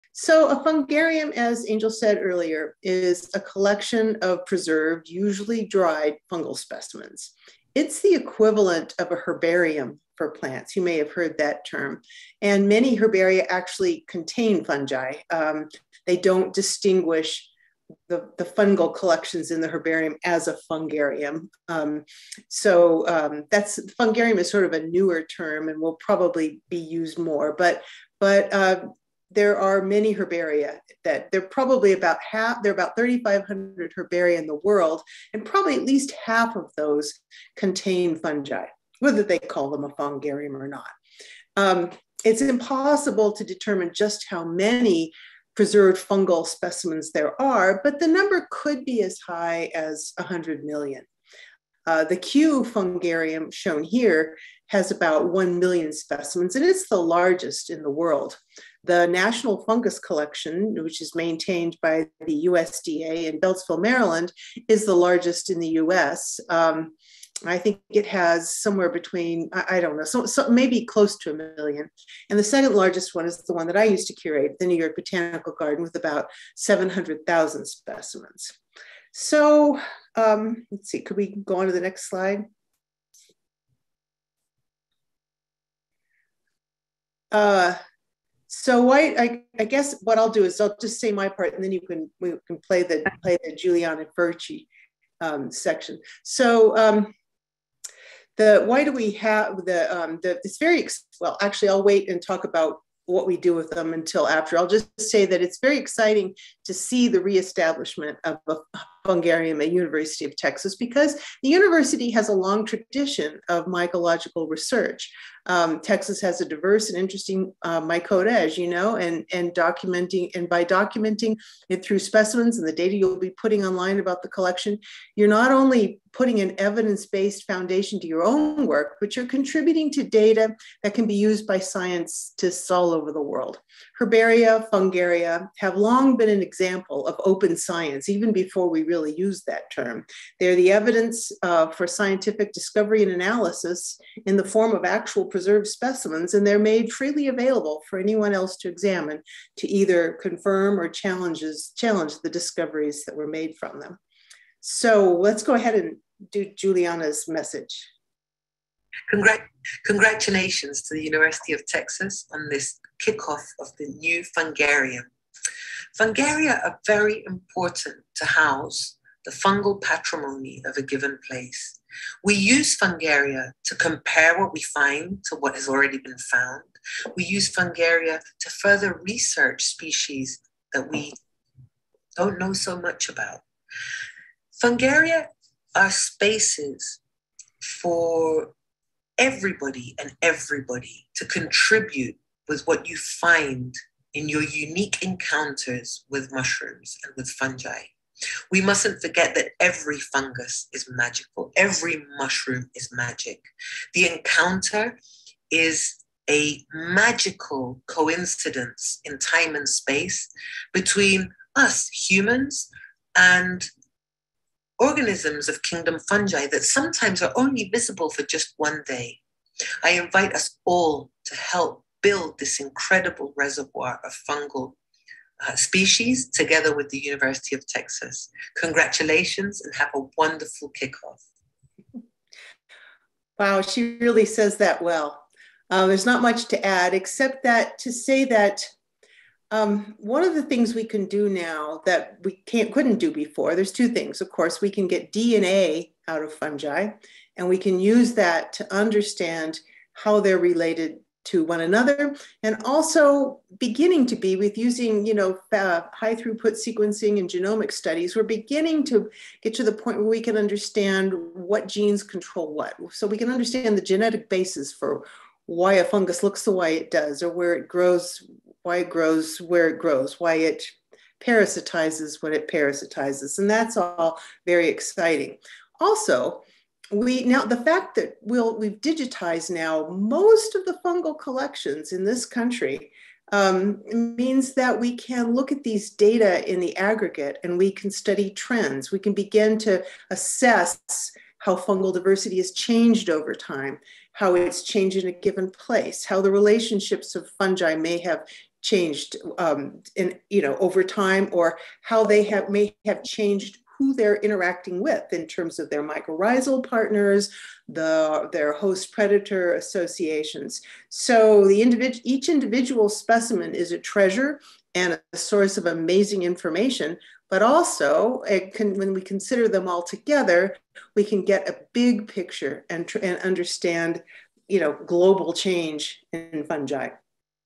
PART 4: Importance of Fungarium w/ Dr. Barbara Thiers & Giuliana Furci
Join a panel of mycologists and enthusiasts to learn about the importance of the new fungi collection that will be preserved at UT. Learn how you as a citizen scientist can help with the collection in several ways.

The panel will discuss how fungaria fuel research and teaching by documenting diversity and increasing our understanding of how anthropogenic activities can change species distributions. Currently, UT does not have a permanent fungal collection, or fungarium, but you can help us in the establishment and growth of a new collection.